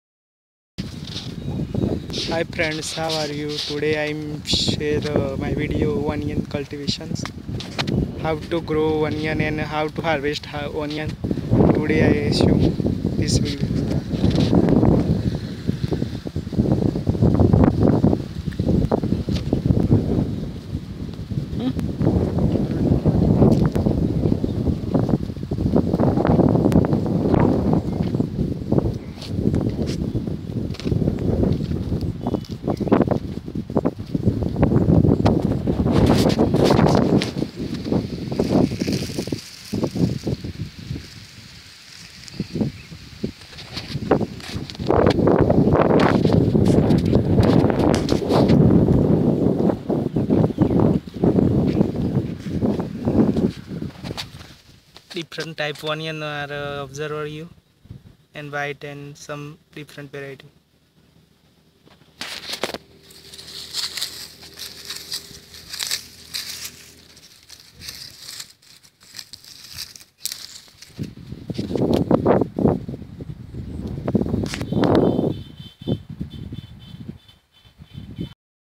cultural practices of onion crop.
Hii friends how are you today i share my new video on onion crop how to harvesting of onion crop and how to care of onion crop.seen in my video.